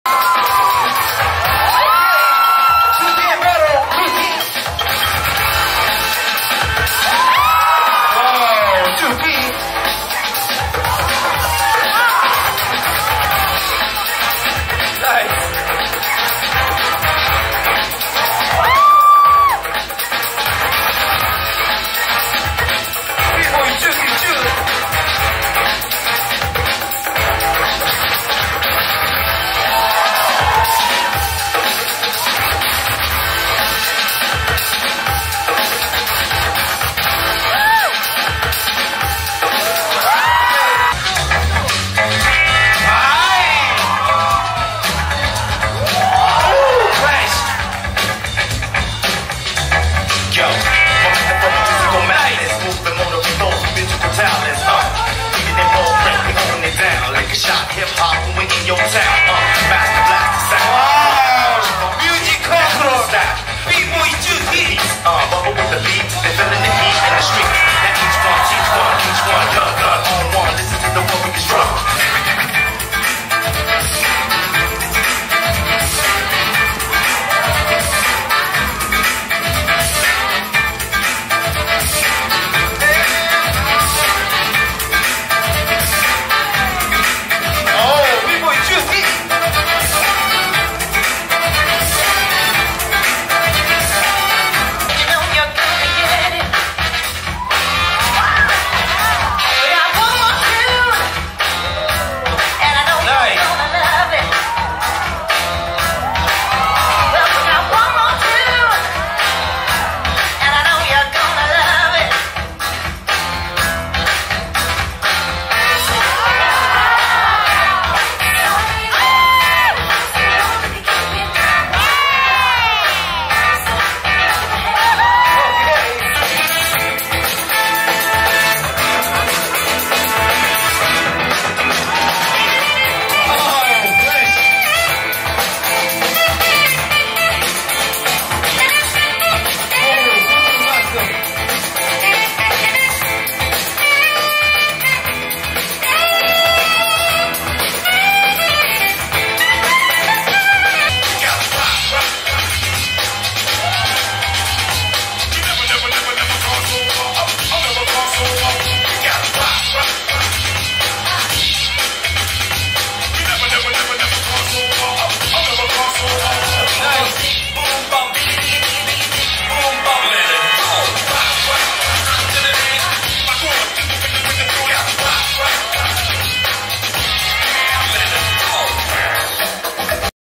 Bboy Zooty Zoot Recap | Champion | Flabor 2023
Bboy Zooty Zooty (Baseus/Seoul Skillz) Recap at Flabor 2023.

Bboy Zooty Zoot The Champion of Flabor 2023 2v2 Breaking Battle with Bboy Vero.